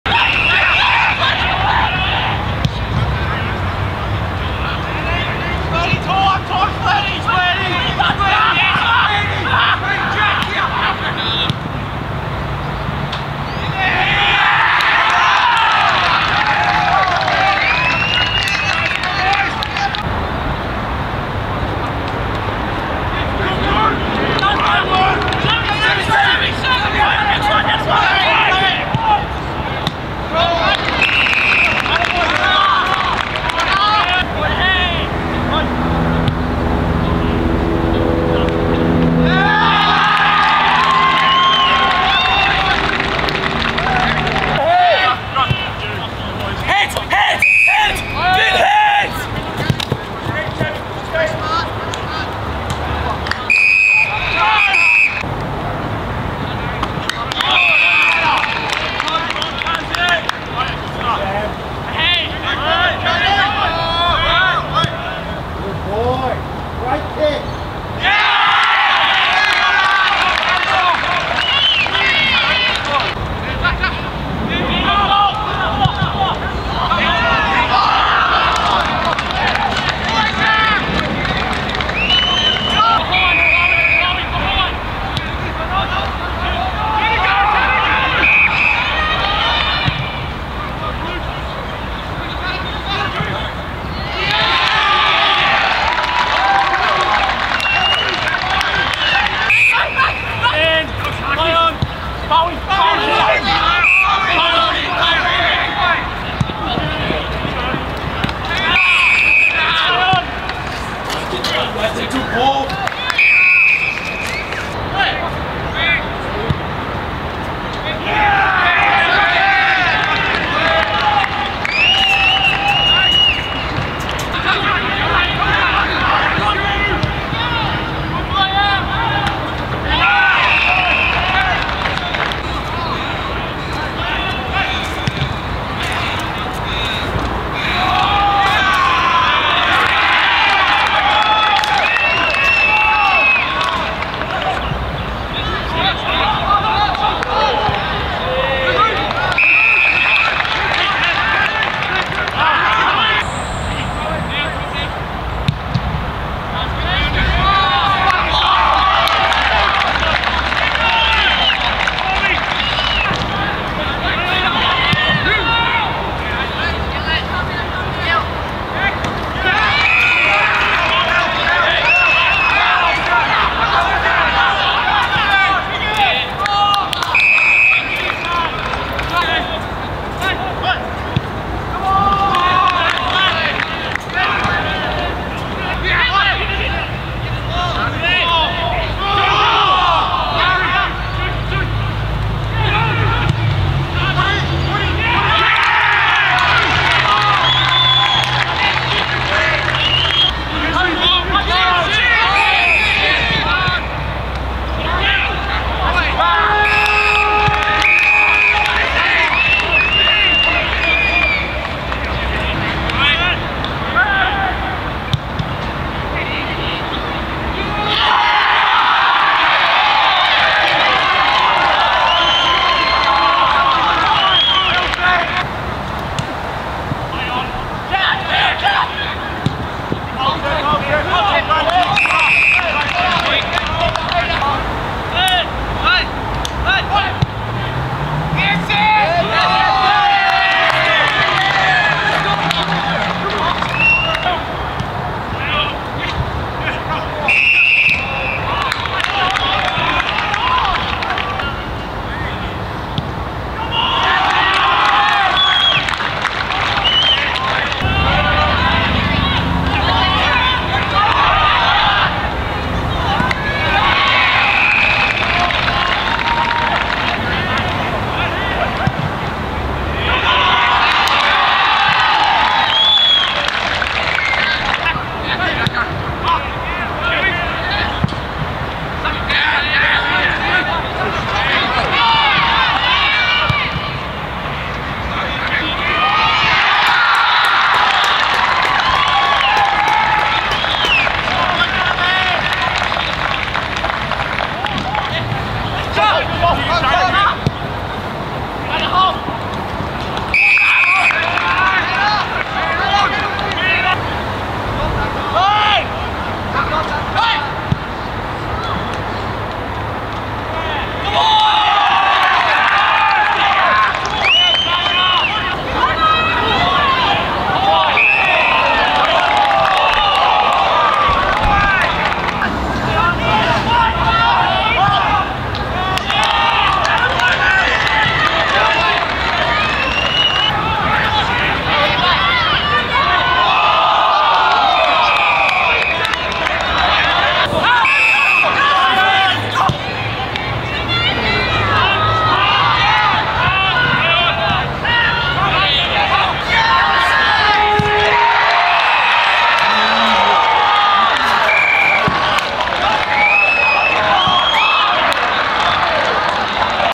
Tasmania Devils v Dandenong Stingrays Prelim Final Highlights | Stingrays Advance Through to Granny
Tasmania Devils:
- Colby McKercher played well in midfield for 23 disposals, showing good acceleration from stoppages
- Seth Campbell was great playing mostly half-forward, earning 20 disposals and adding a goal; he presented well up the ground and was damaging with his ball use
- Jack Callinan was also impressive in the front half, displaying strong hands in the air and setting up teammates in better scoring positions; finished with a goal and 18 disposals

Dandenong Stingrays:
- Jaxon Binns was arguably best afield with his 26 disposals and a goal, using it well on either foot and stepping up in the absence of Mitch Szybkowski
- Kade De La Rue was fantastic playing a mix of midfield and forward; gathering 20 disposals and kicking a goal
- Cooper Simpson rotated through the midfield and forward line and was productive for his 16 disposals and 2 goals, both of which were top class finishes